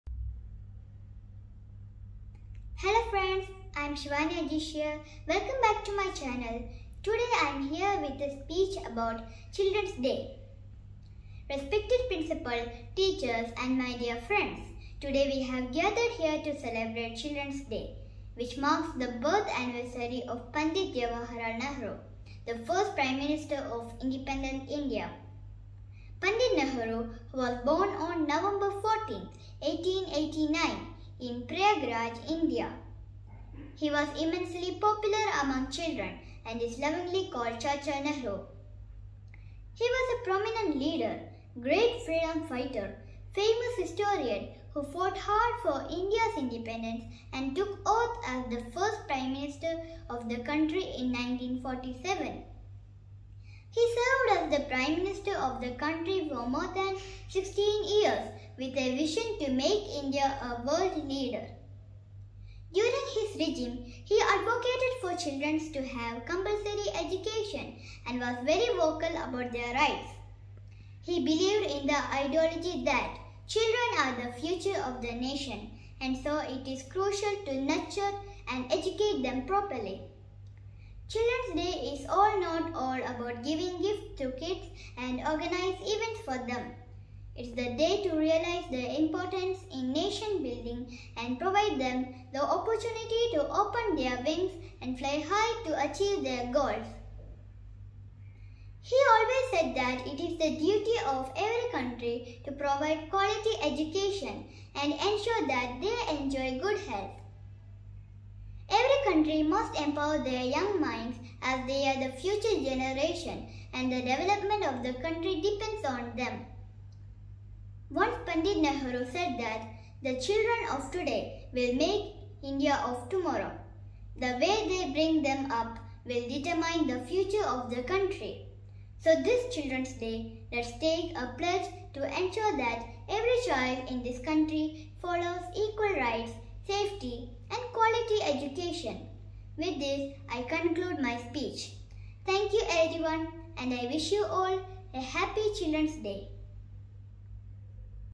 SHORT SPEECH ABOUT CHILDREN'S DAY AND PANDIT JAWAHAR LAL NEHRU
This video is a short speech about Children"s Day and Chacha Ji.